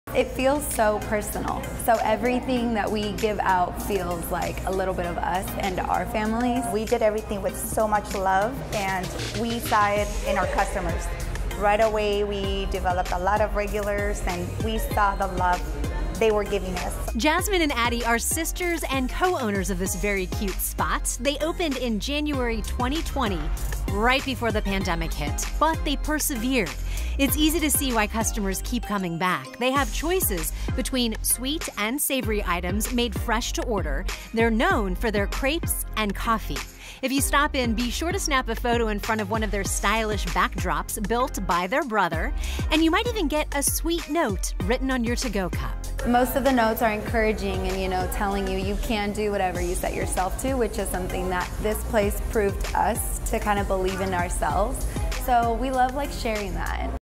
Kent café specializes in coffee, crepes, and sweet affirmations
In a world of negative internet comments, a small café in Kent is dedicated to sharing niceties.

Sweet Notes Café specializes in crepes, coffee, and writing affirmations on to-go cups.

"Most of the notes are encouraging, telling you you can do whatever you set yourself to — which is something that this place proved to us, to kind of believe in ourselves," said Jasmin Mendoza who owns Sweet Notes with her sister Addie. "We love sharing that."

They opened in late January, 2020 — right before the pandemic hit. The first year was especially difficult, but they persevered. Addie credits their customers.

"We did everything with so much love, and we saw it with our customers,” she said. “Right away, we developed a lot of regulars and we saw the love they were giving us."

The café features pink velvet chairs, marble tables, and multiple selfie walls for Instagram photos (built by the Mendoza’s brother.)

Everything on the menu is made fresh to order. There are sweet and savory breakfast and lunch options, including acai bowls, avocado toast, and crepes. Customers can also order from a full espresso bar.

"It feels so personal, so everything we give out feels like a little bit of us and our family,” Jasmin said.

Sweet Notes Café is located at 12966 S.E. Kent-Kangley Rd. and is open seven days a week for breakfast and lunch, from 8 a.m. – 3:30 pm.